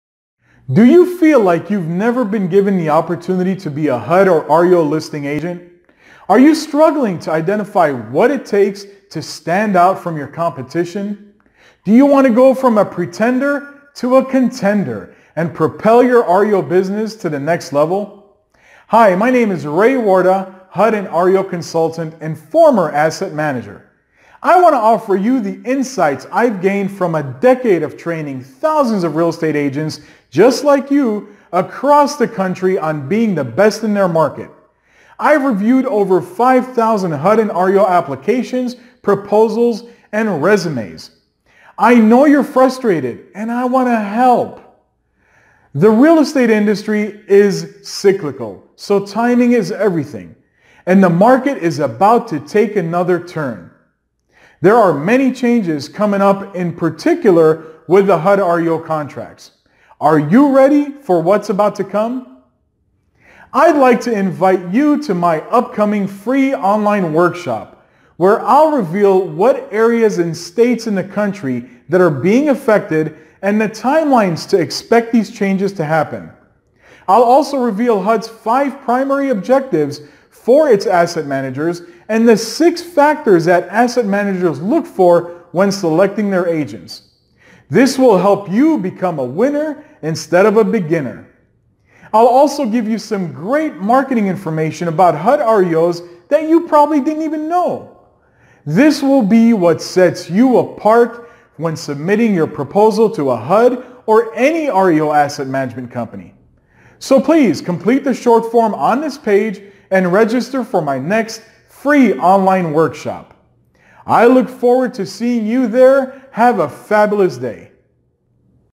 HUD REO FREE Webinar - Ray Warda
Do you feel like you've never been given the opportunity to be a HUD or REO listing agent? Are you struggling to identify what it takes to stand out from your competition?

Do you wanna go from being a pretender to a contender and propel your REO business to the next level?

Hi, my name is Ray Warda HUD and REO Consultant and former Asset Manager.  I wanna offer you the insights I've gained from a decade of training 1000s of Real Estate agents just like you, across the country on being the best in their market. I've reviewed over 5,000 HUD and REO applications, proposals and resumes.

I know you're frustrated and I want to help.

The Real Estate industry is cyclical so timing is everything, and the market is about to take another turn.

There are many changes coming up in particular with the HUD REO contracts. Are you ready for what's about to come?

I'd like to invite you to my upcoming FREE online workshop where I'll reveal what areas and states in the country that are being affected and the timelines to expect these changes to happen.  I'll also reveal HUD's 5 primary objectives for its Asset Managers and the 6 factors that asset managers look for when selecting their agents. This will help you become a winner, instead of a beginner.

I'll also give you some great marketing information about HUD REO's that you probably didn't even know! This will be what sets you apart when submitting your proposal to a HUD or any REO Asset Management company.  So, please complete the short form on this page and register for my next FREE online workshop.

I look forward to seeing you there.  Have a fabulous day!